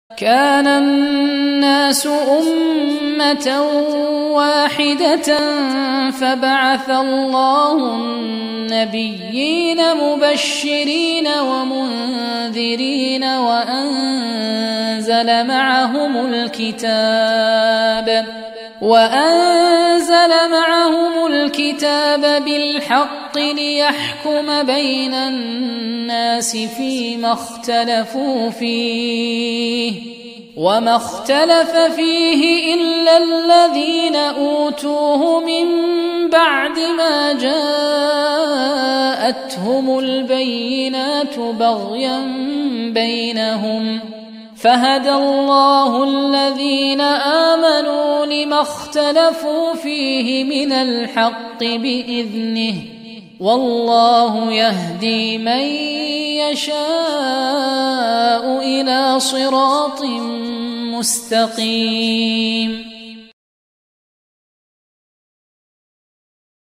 "Mankind was [of] one religion [before their deviation]; ..."
Mankind was [of] one religion [before their deviation]; then Allah sent the prophets as bringers of good tidings and warners and sent down with them the Scripture in truth to judge between the people concerning that in which they differed. And none differed over it [i.e., the Scripture] except those who were given it – after the clear proofs came to them – out of jealous animosity among themselves. And Allah guided those who believed to the truth concerning that over which they had differed, by His permission. And Allah guides whom He wills to a straight path.